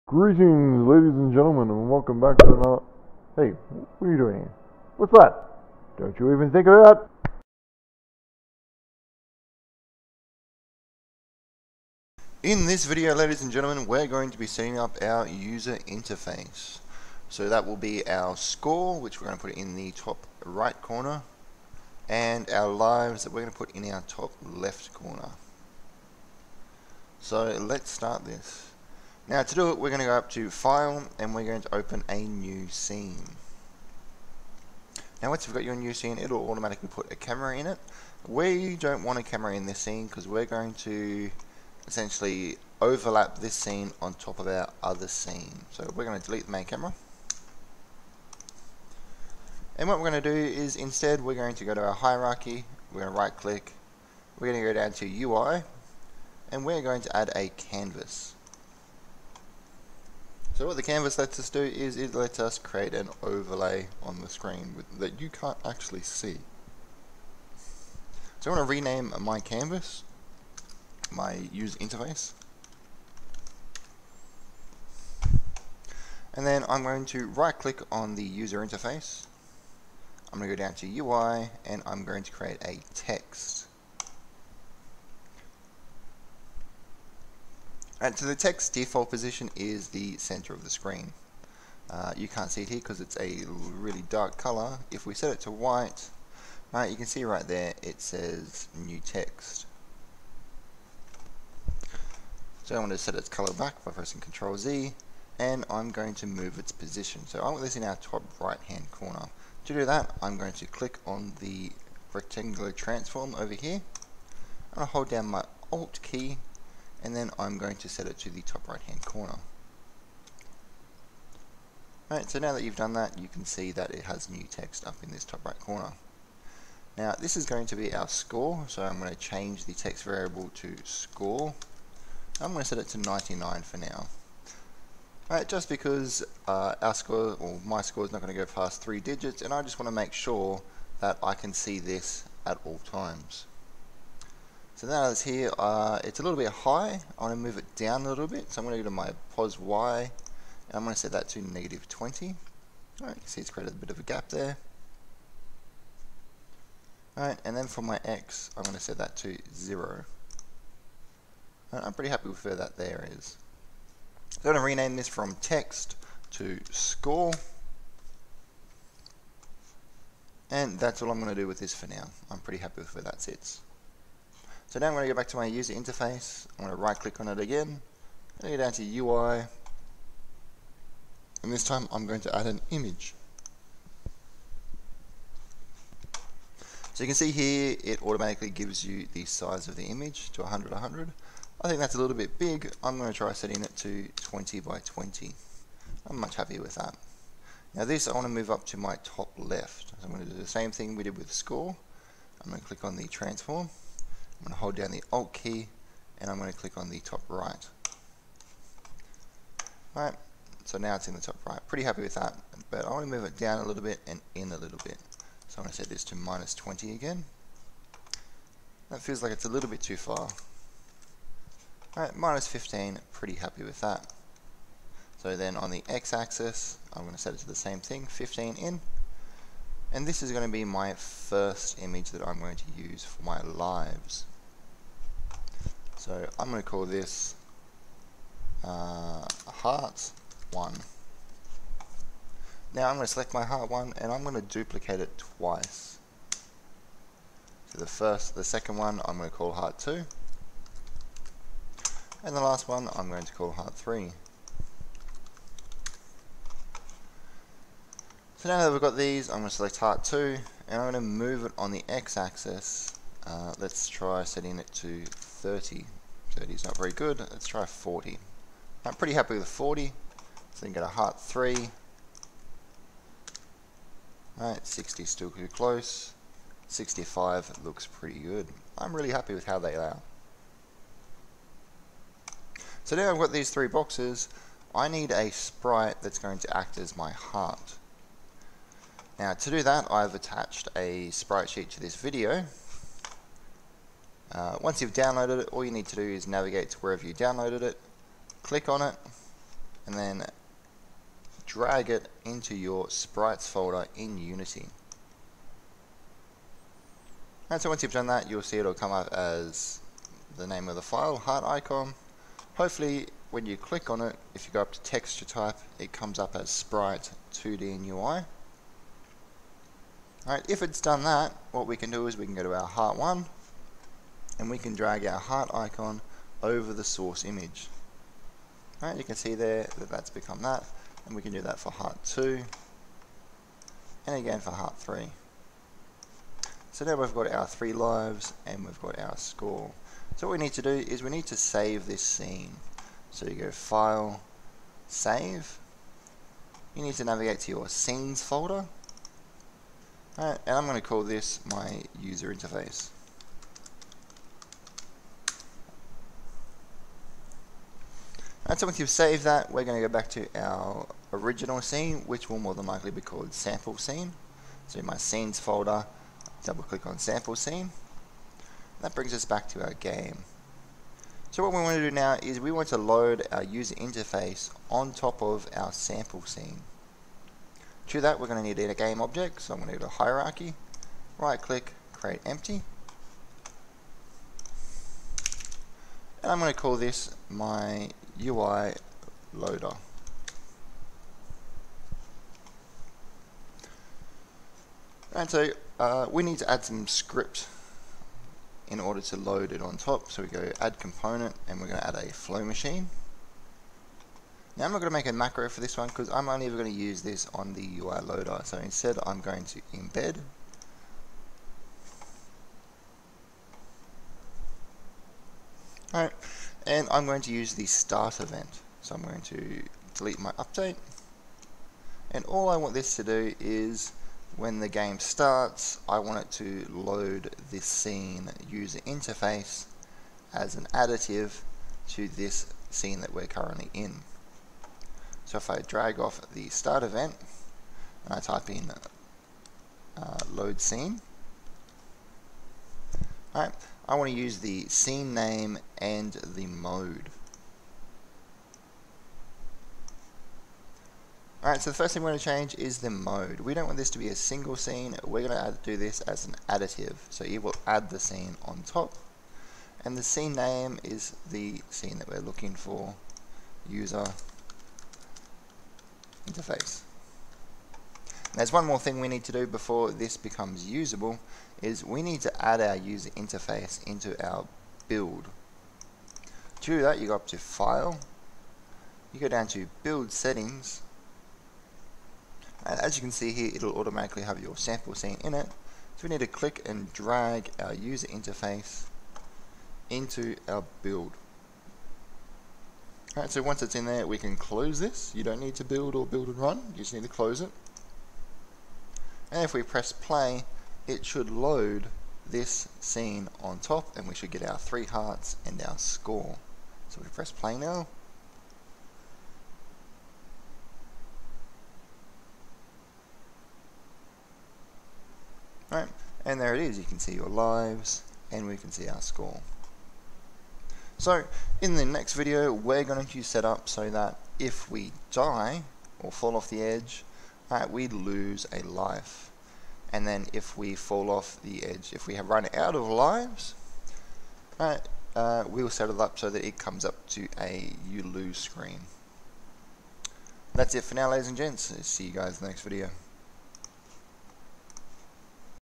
Introduction to Bolt 07
In this video, we will set up our UserInterface scene and a UILoader to import the scene into our game.